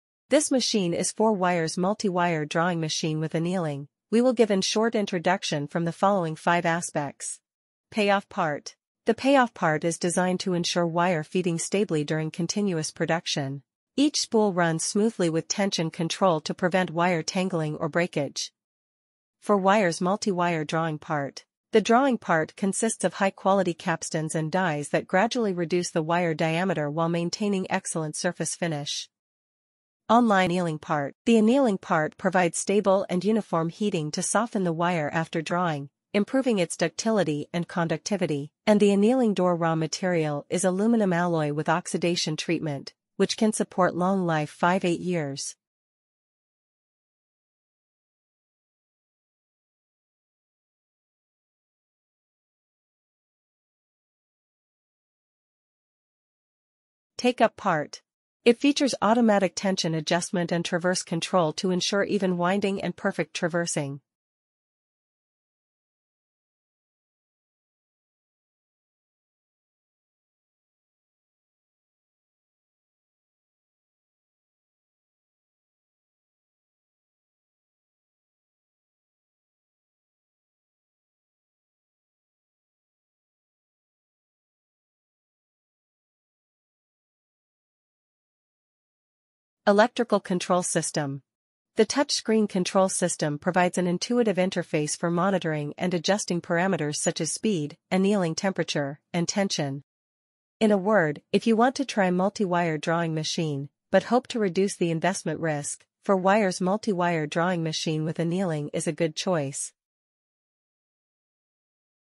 Multiwire Drawing Machine for 4 Wires
If you intersted in the multi wire drawing, but don't want to inverst too much. This Multi Wire Copper Wire Drawing Machine is a good choice. It is mainly used for drawing copper wire from 1.2mm to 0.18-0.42 mm wire.